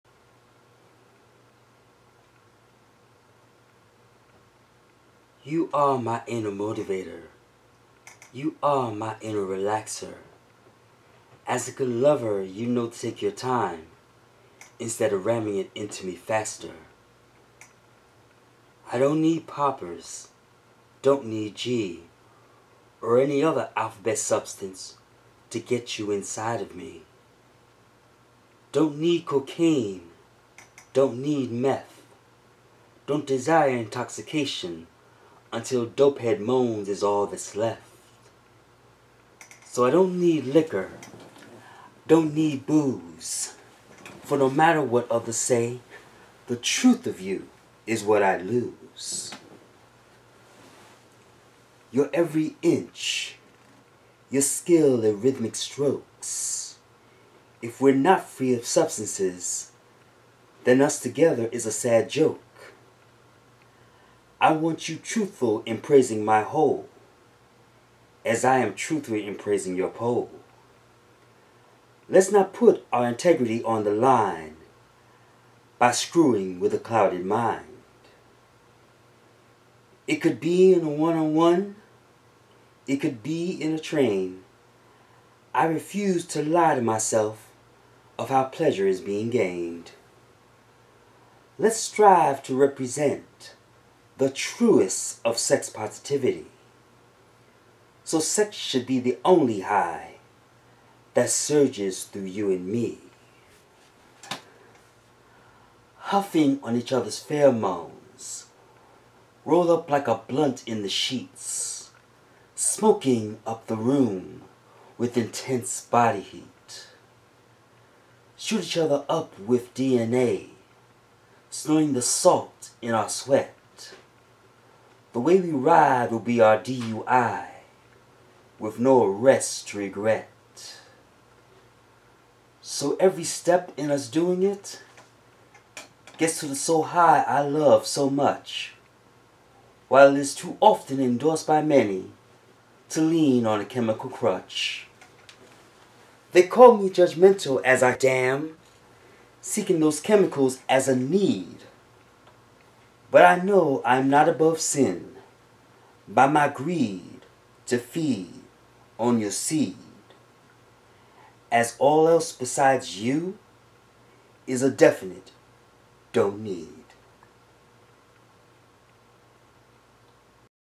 Don't Need
There are many who feel they need substances in order to have sex. For some it's weed, alcohol, or even worse (and illegal) substances like poppers, cocaine, crystal meth, GHB, etc. Sadly, this is more so the case in the gay community. So this poem is for to be a voice for those who knows and live by the sentiment of how those things are a don't need for sex.